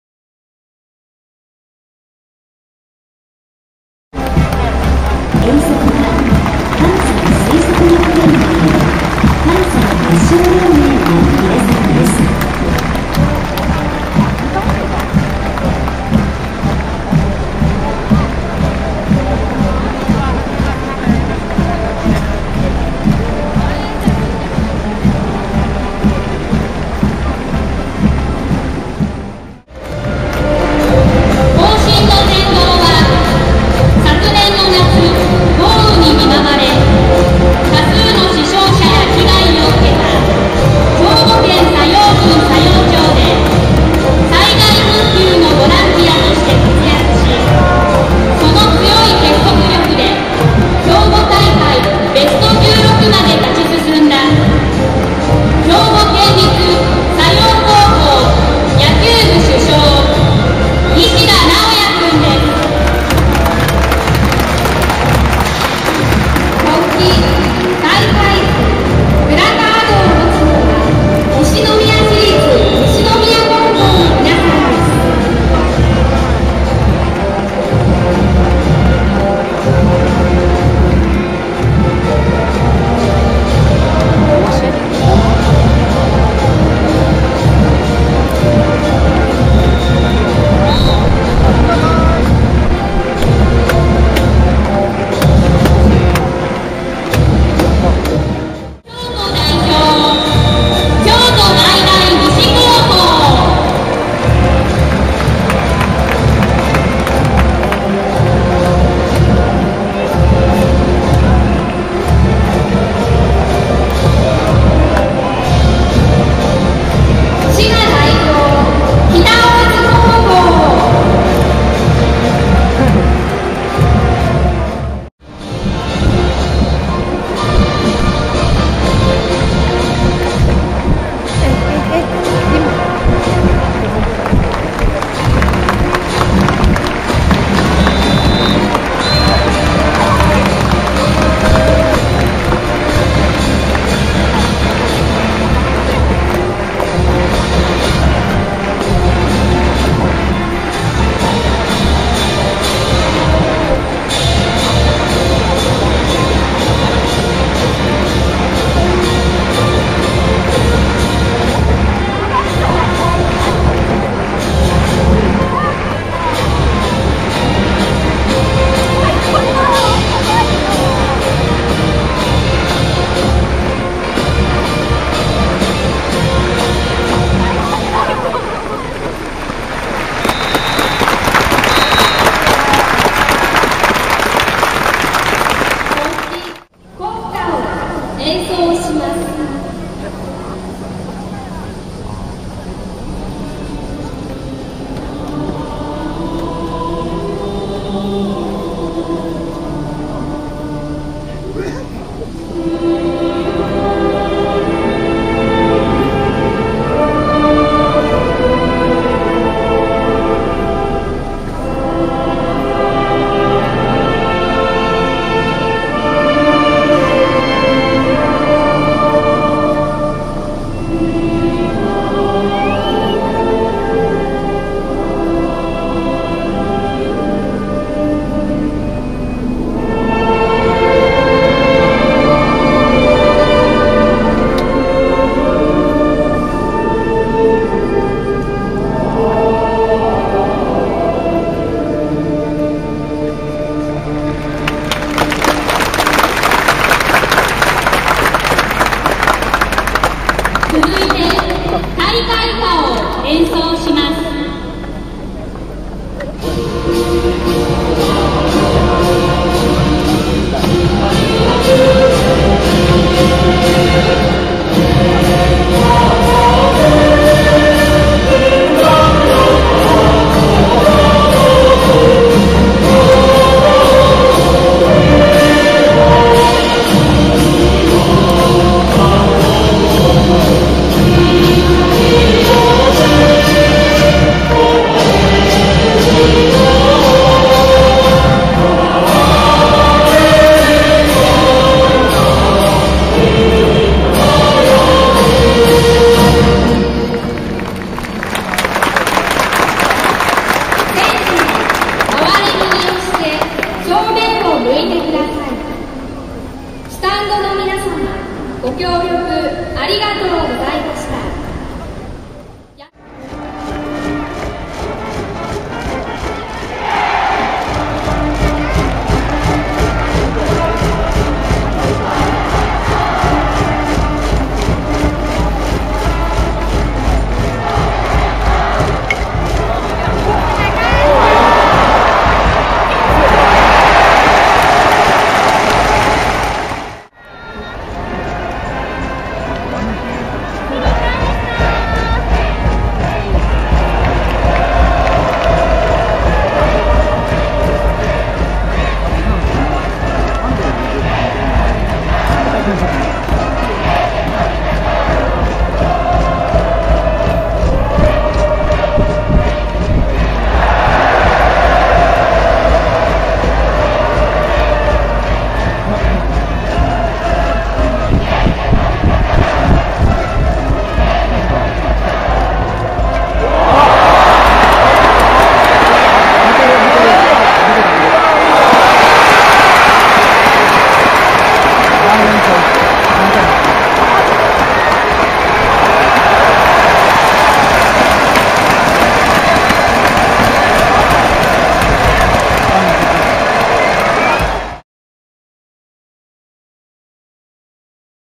Koshien opening day
August 7th was the opening day of the 92nd National High-School Baseball Tournament, taking place in Koshien.

The picture quality is poor, I know, but this should nonetheless give you an idea of the atmosphere there.
Pictures taken with a proper camera have been uploaded separately, and are available